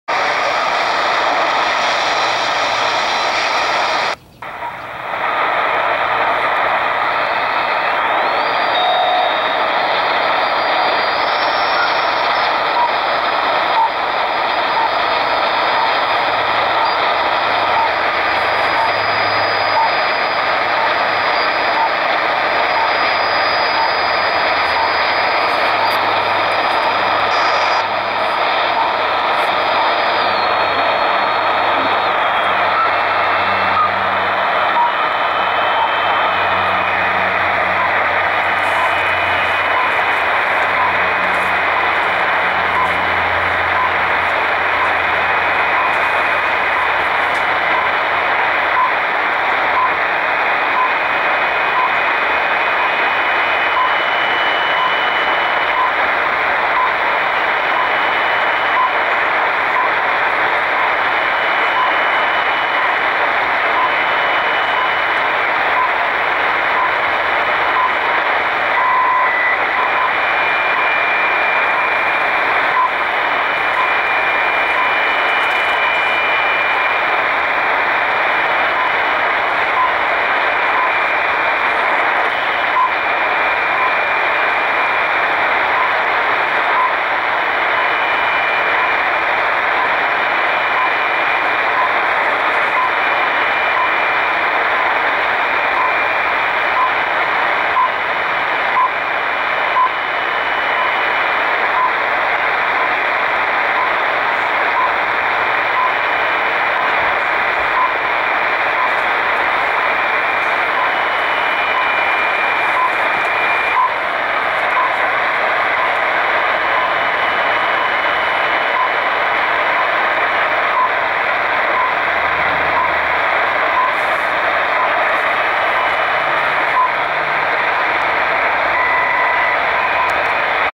RWM Moscow time pips on 9996 khz Tecsun pl-600
Moscow time signal pips on 9996 khz on tecsun portable receiver on may 7th 2013